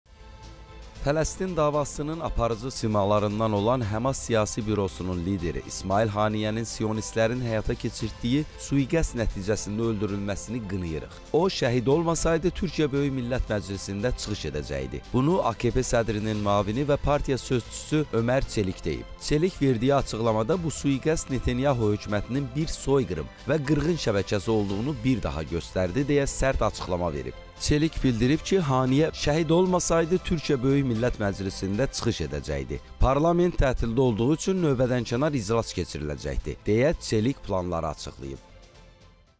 O, şəhid olmasaydı… | AKP sədrinin müavinindən sərt açıqlama
“Fələstin davasının aparıcı simalarından olan HƏMAS Siyasi Bürosunun lideri İsmayıl Haniyənin sionistlərin həyata keçirdiyi sui-qəsd nəticəsində öldürülməsini qınayırıq”.
Bunu AKP sədrinin müavini və partiya sözçüsü Ömər Çelik deyib.
Ö.Çelik verdiyi açıqlamada “Bu sui-qəsd B.Netanyahu hökumətinin soyqırımı və qırğın şəbəkəsi olduğunu bir daha göstərdi”, – deyə sərt açıqlama verib.
O bildirib ki, İ.Haniyə şəhid olmasaydı, Türkiyə Böyük Millət Məclisində çıxış edəcəkdi.
“Parlament tətildə olduğu üçün növbədənkənar iclas keçiriləcəkdi”, – deyə Ö.Çelik planları açıqlayıb.